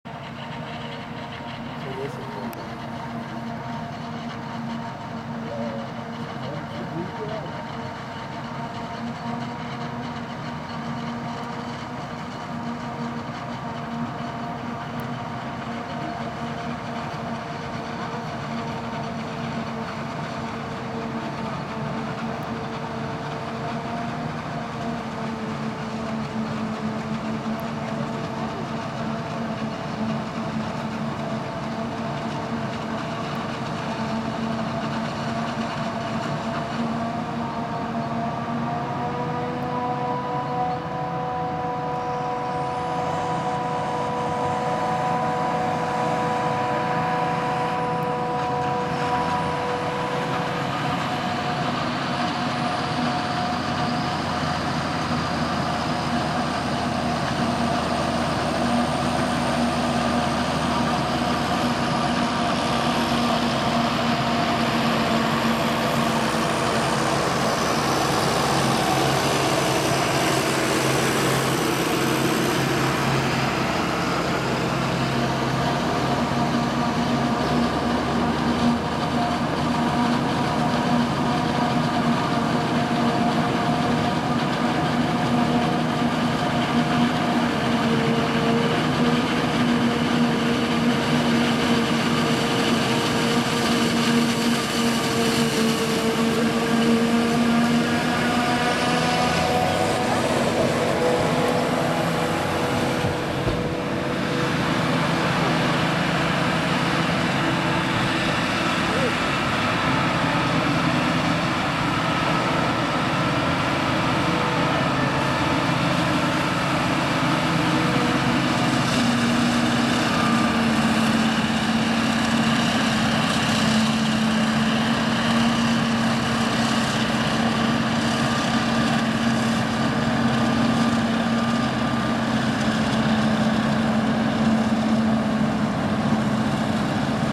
Mais 2011 : Egide London met Claas Jaguar 900 12 rijer en 2x John Deere 7530 in Aubel
Mais 2011 : Als je met 12 rijen hakselt zijn de tandems zo vol.. gelukkig zijn er genoeg combinaties op voorraad.. De 1e John Deere 7530 met tandem Joskin is vol en word afgewisseld door een John Deere 7530 met Schuitemaker silagewagen.. mooie machines hier in Aubel..